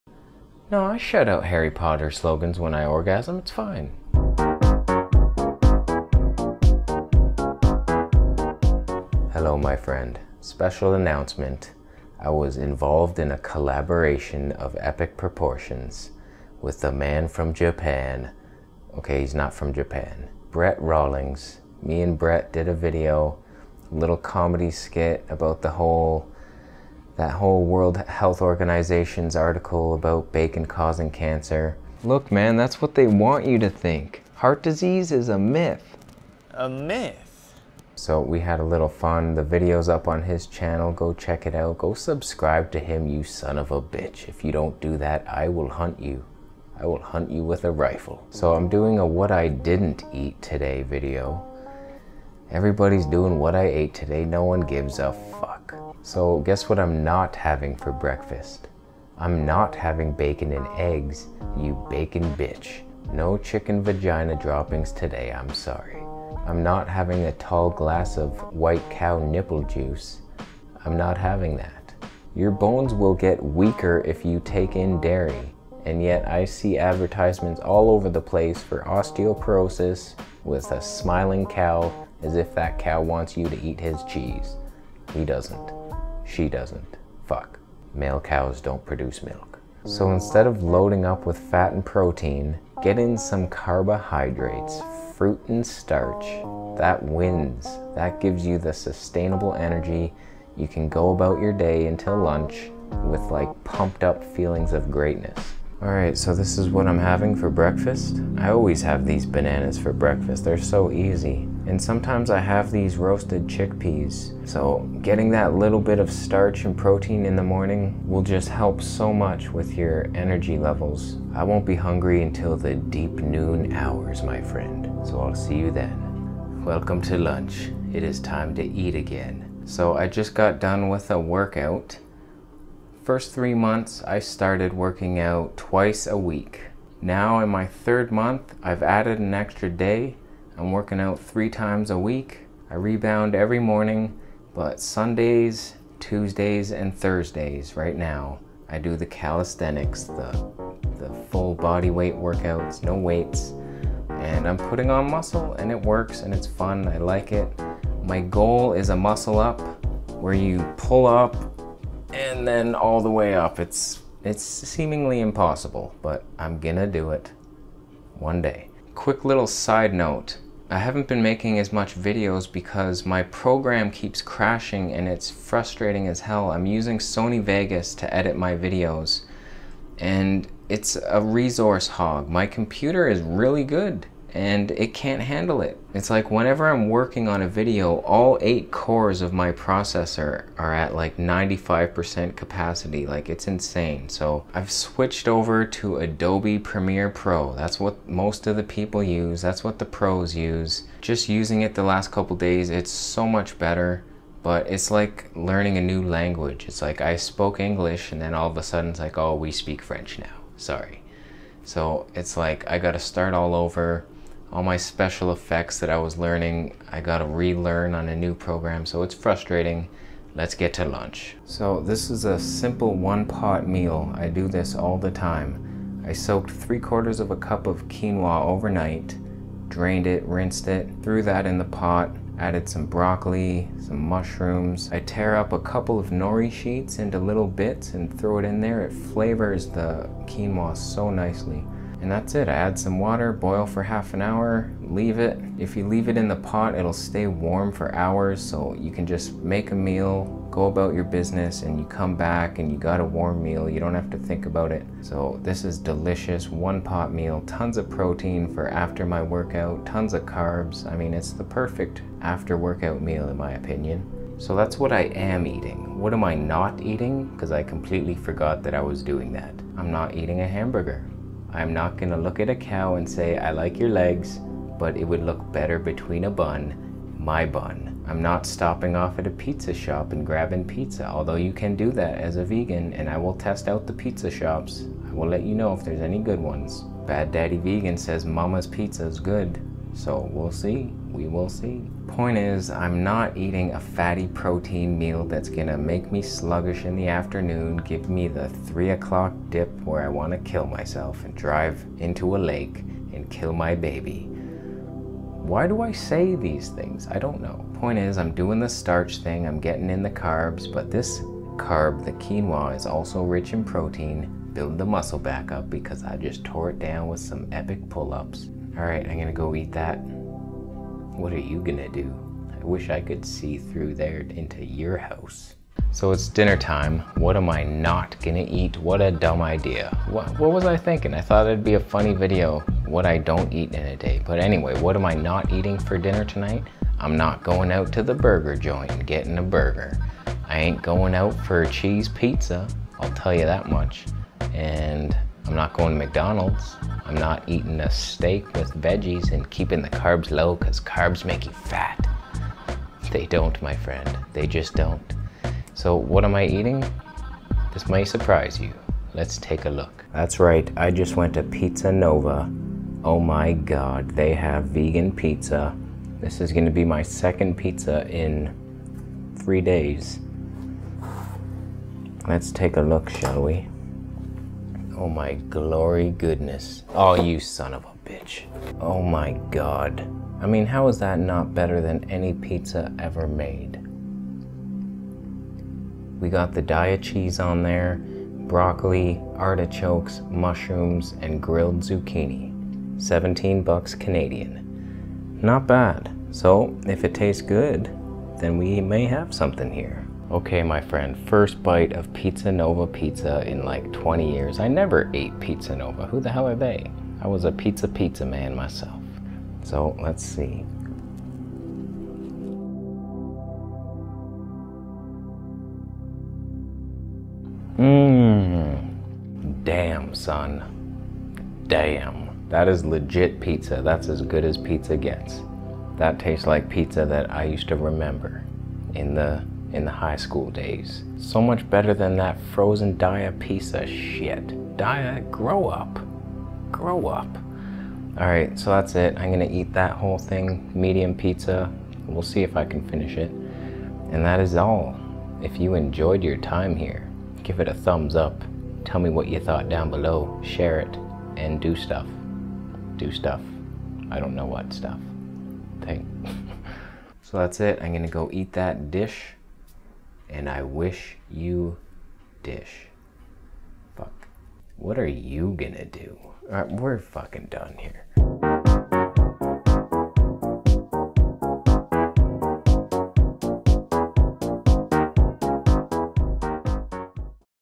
What to eat and NOT eat to be healthy. Plus SPECIAL ANNOUNCEMENT!
There are some healthy foods to eat, and unhealthy ones.  I lay the hammer down and banish some foods to hell where Jesus vacations.

Here is the video Brett Rawlings and I made! It's a short comedy skit and I think you will really enjoy it!  It's a parody of the World Health Organization's article about processed meats like bacon and hot dogs causing cancer.  We got plenty of more videos coming!

Today's video is a what I didn't eat in a day video, combined with a what I did eat today video.  All edited for the first time in Adobe Premiere Pro.  I've switched over from Sony Vegas, so be patient with me as I learn this new program!

These are some of my favourite videos:

Funniest Videos

Entertaining and good reasons to eat plant based